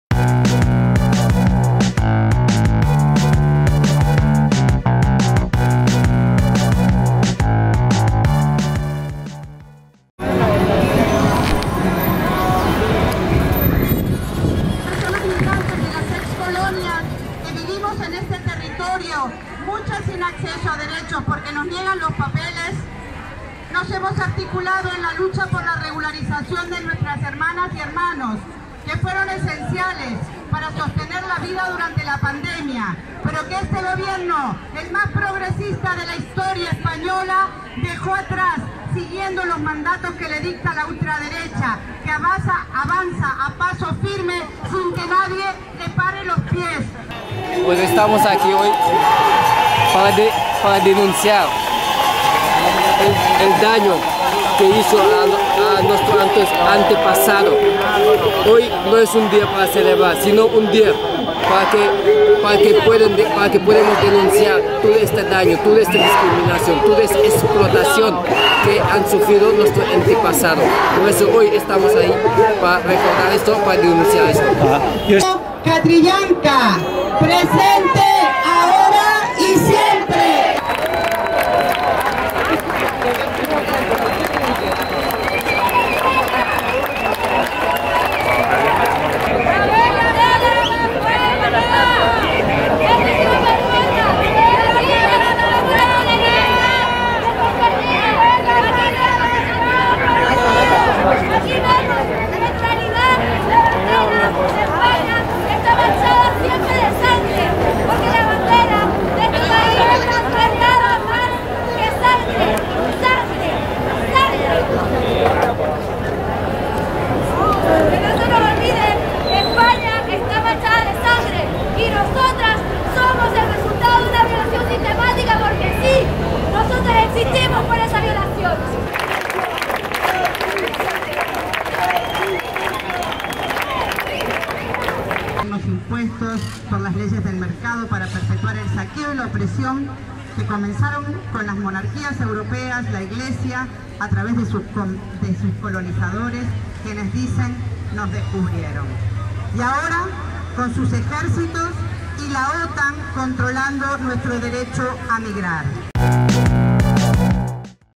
Barcelona 12 octubre 2022: Descolonización, Regularización ya!
Barcelona 12/10/2022.- Convocatòria contra el colonialisme i la celebració del dia de l'Hispanidad i per la regularització de les persones migrades que se li neguen els drets.